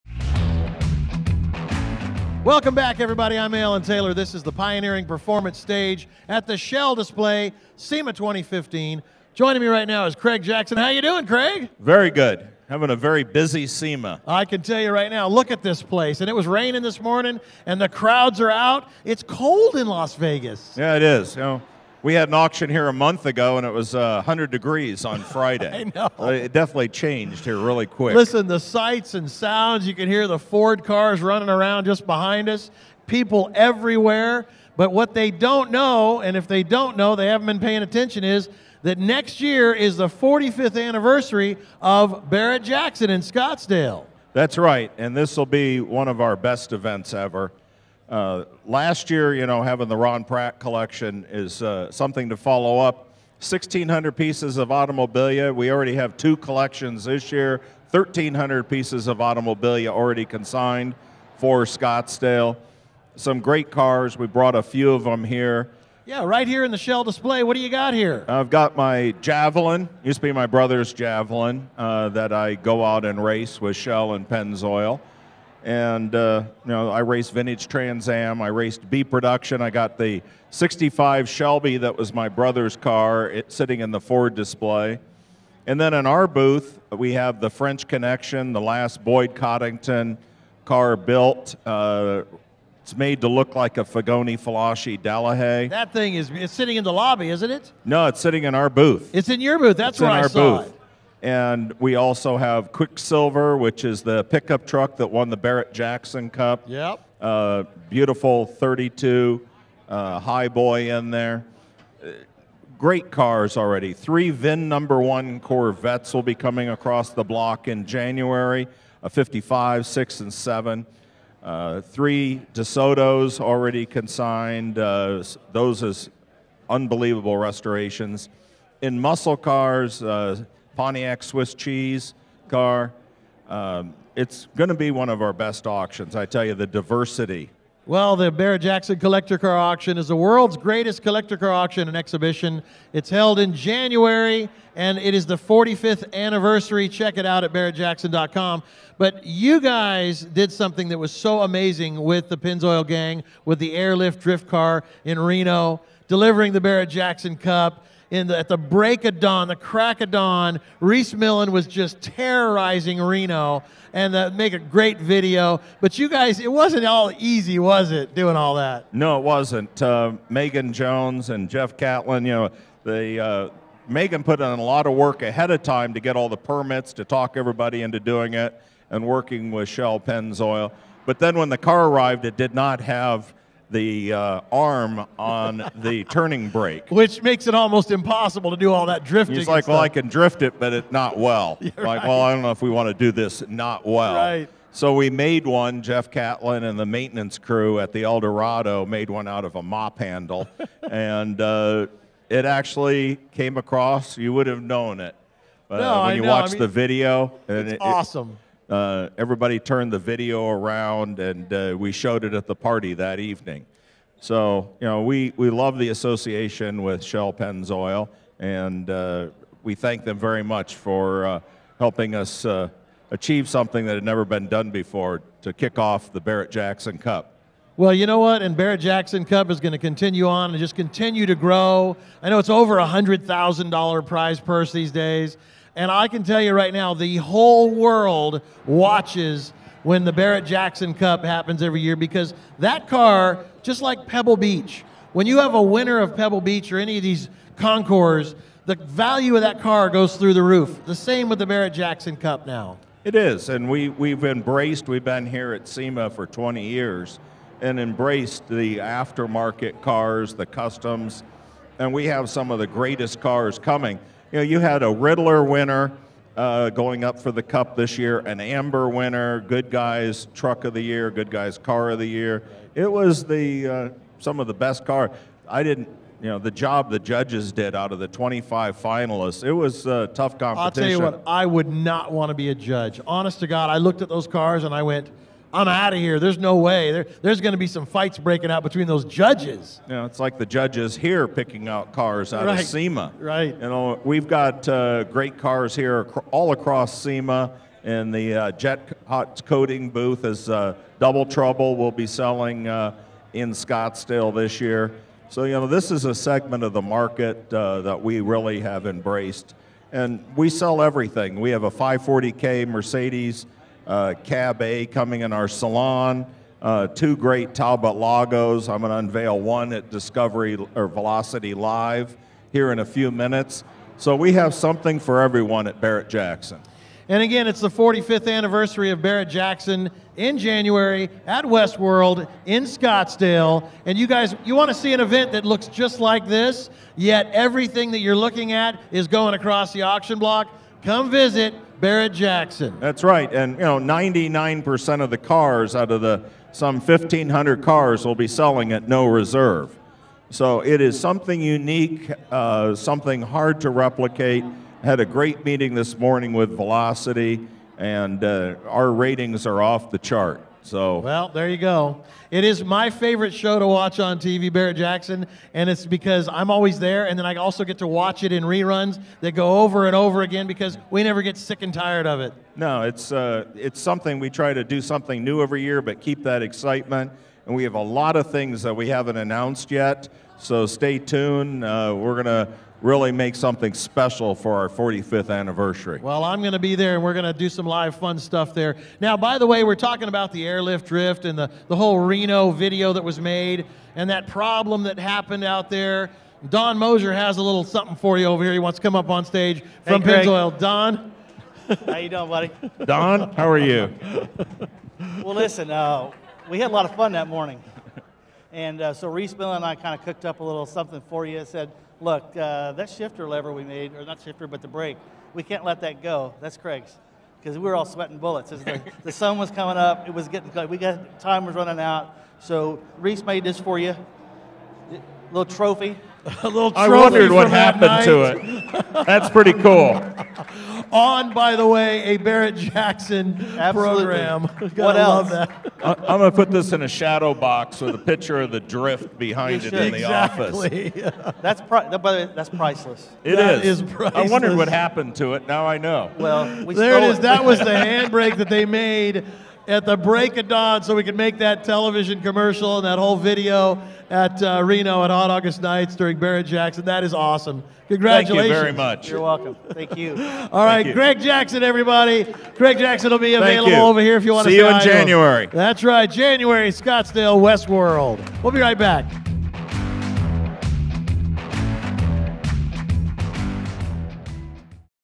Craig Jackson of Barrett-Jackson at the Shell SEMA Experience
Craig Jackson talks with Alan Taylor at SEMA 2015 about the amazing line-up of cars as Barrett-Jackson Collector Car Auction gears up for its 45th anniversary next January in Scottsdale. Don Moser (Marketing Operations Team Lead, Shell Lubricants) presents Craig Jackson with a surprise trophy from Rhys Millen!